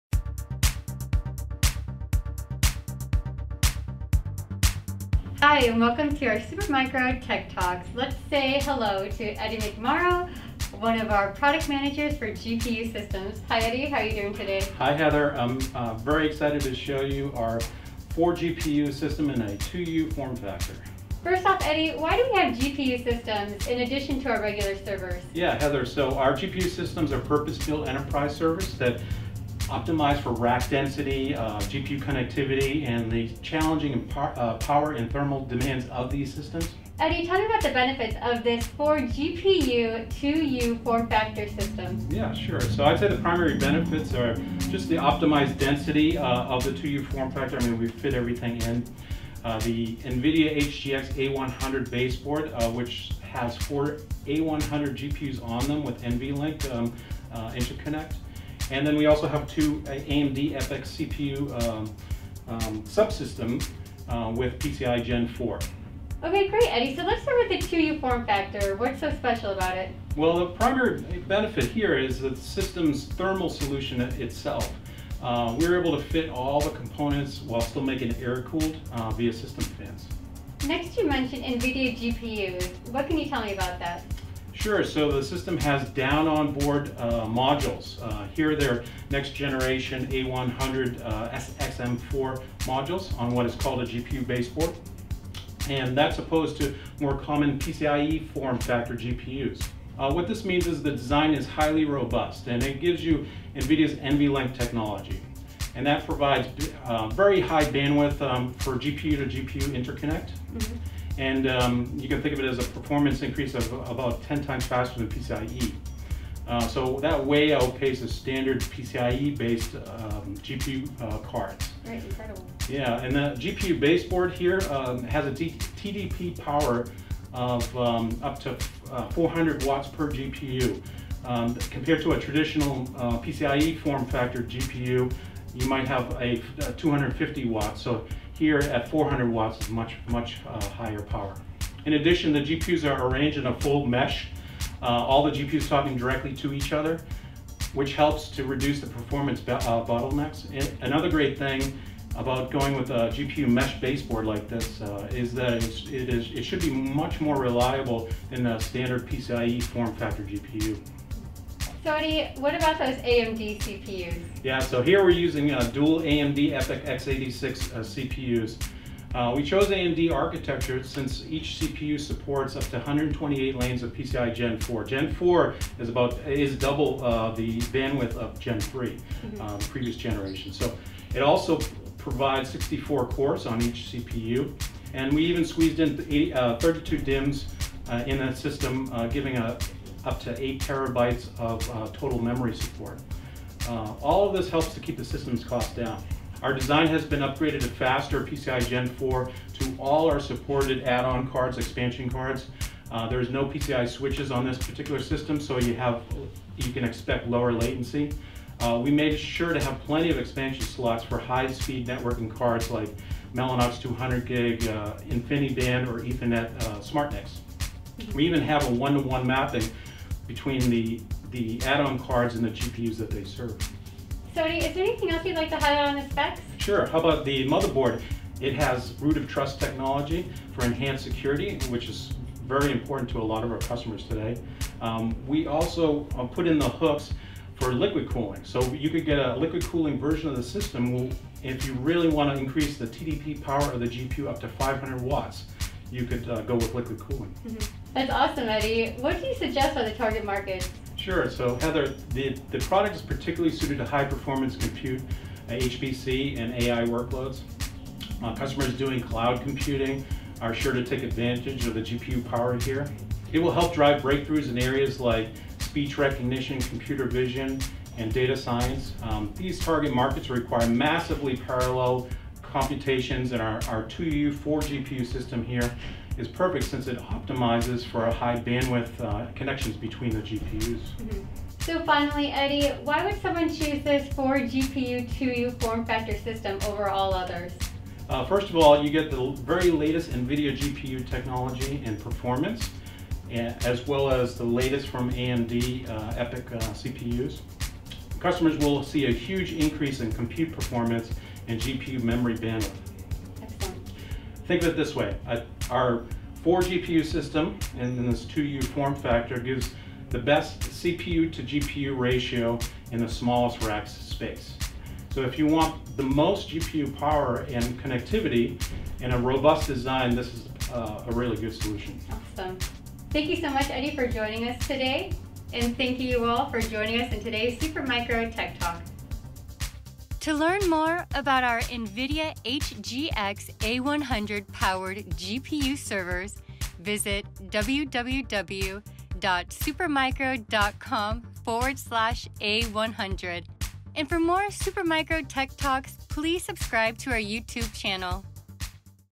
Supermicro Tech Talks - Episode 1
In the first episode of our new Supermicro Tech Talks series, we take a look at our 2U system featuring the NVIDIA HGX A100 4 GPU board with four direct-attached NVIDIA A100 Tensor Core GPUs using PCI-E 4.0 for maximum performance and NVIDIA NVLink™ for high-speed GPU-to-GPU interconnects.